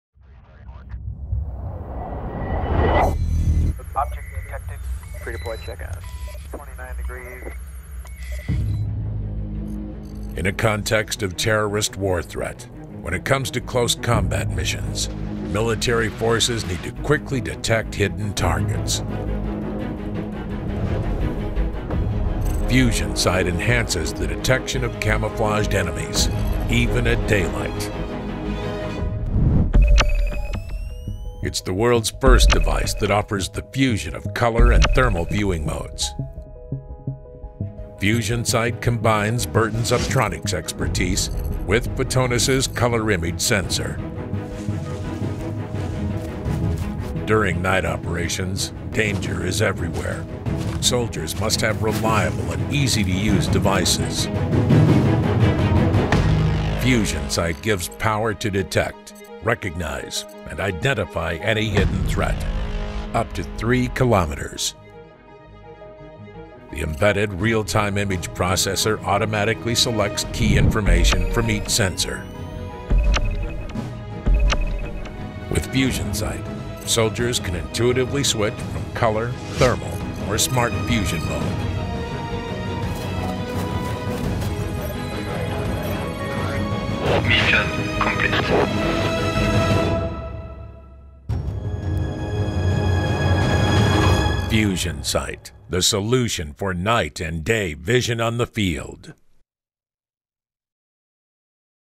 FusionSight Digital Night Vision Device
Bertin Instruments launches the first digital Night & Day Vision Device providing smart fusion for accurate threat and target detection.

Bertin Instruments, monitor the invisible